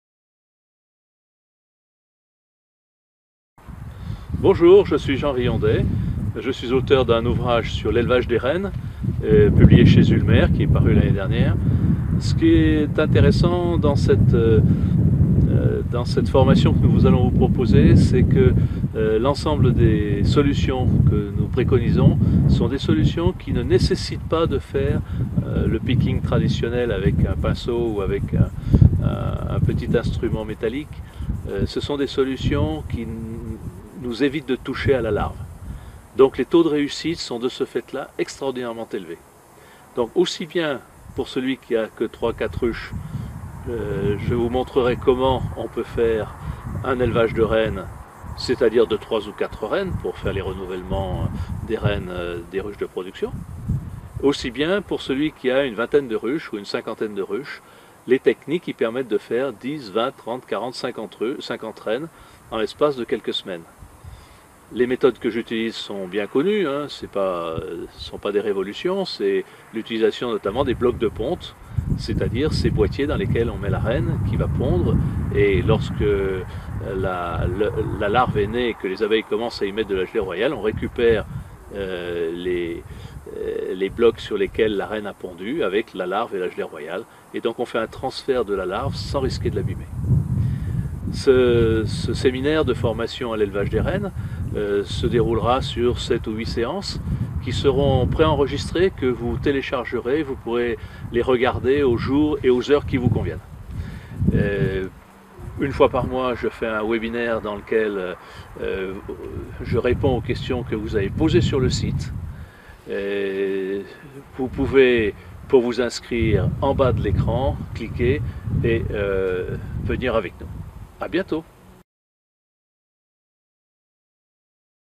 Formation apiculture : l'élevage des reines simplifié.
Présentation par Jean Riondet de la formation à l'élevage des reines simplifié.

Jean Riondet est apiculteur et auteur de plusieurs livres d'apiculture, notamment "Elever ses reines, 3 méthodes simples pour réussir à coup sûr" paru chez Ulmer.

La programme s'articulera autour de ces parties

1ère partie :
Pourquoi élever des reines ?
Pour avoir toujours de jeunes reines qui sont les meilleures pondeuses mères de grandes populations. Comment produire des reines de qualité ?
La sélection ? Produire quelques reines, la scission des colonies, l’essaimage artificiel.
Faire des essaims quels objectifs ?
Faire des essaims pour la production de reines avec un minimum de matériel biologique, l’essaim sur un seul cadre.

2ème partie :
Quel est le principe de l’élevage, comment réunir les conditions de production des reines ?
Le déroulé de l’élevage de l’œuf à la reine, le matériel, la constitution du starter ouvert ou fermé, de l’éleveuse, de la couveuses, cas de la couveuse australienne.
Comment constituer les nucléis ou essaims artificiels. L’élevage artificiel de reines, le processus classique du transfert des larves avec un picking, pourquoi l’usage des blocs de ponte ?

3ème partie :
Mise en œuvre des blocs de ponte et l’utilisation des larves d’un jour
Avec starter
- 1 l’élevage à partir d’une ruche divisible, préparation de la colonie, la division, le starter ouvert, l’utilisation des cellules à 3 jours pour faire une vingtaine de reines en une seule fois
- 2 le starter de type Guth. Objectif :  produire un grand nombre de reines en continu

Sans starter : dans des essaims artificiels sur 2 cadres

4ème partie :
Les nucléis ou essaims artificiels leur production leur conduite, l’utilisation des reines, le remérage d’une ruche de production, peut-on sauver une ruche bourdonneuse ?
Comment introduire des reines vierges ?